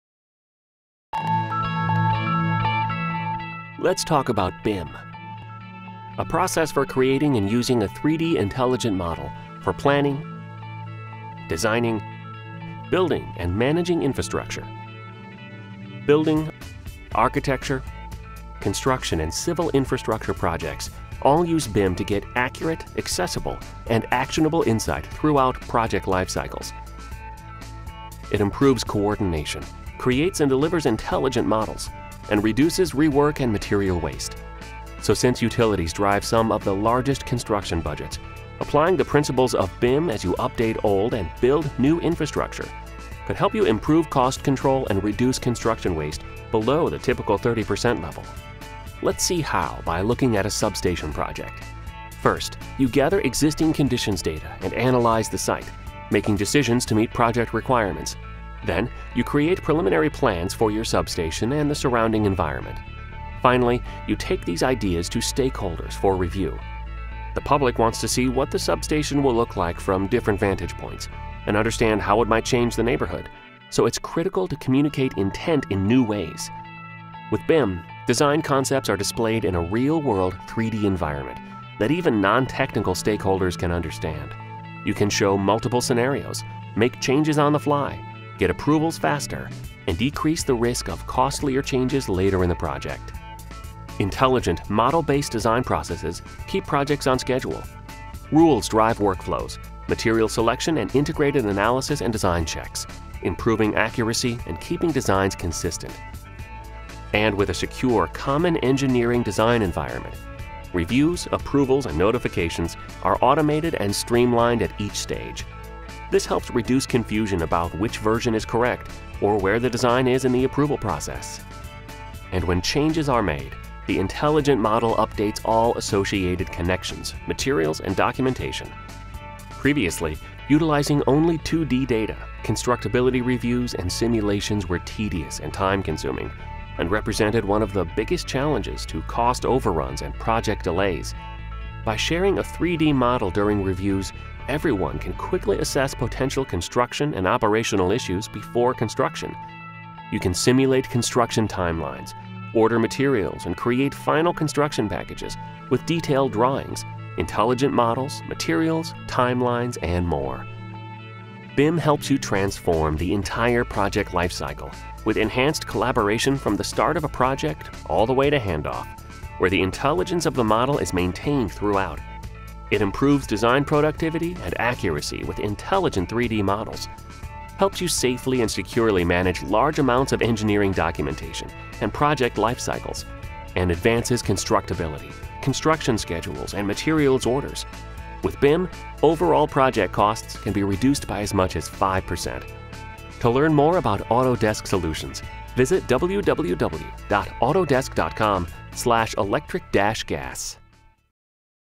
BIM for Utilities
In a variety of other industries, building information modeling (BIM) has been used to address many issues
in the design process, reducing prototyping costs, enabling on-the-fly design changes, and improving
efficiency in materials, labor and scheduling. BIM enables users to more easily update traditional design
standards, documentation procedures, data capture, and data-sharing policies. The adoption of BIM
best practices can help address many of the problems associated with an increase in designer workload
and the integration of new technologies through the creation of intelligent, computational 3-D models.
In utility industry settings, BIM principles can improve planning, design, construction and handoff.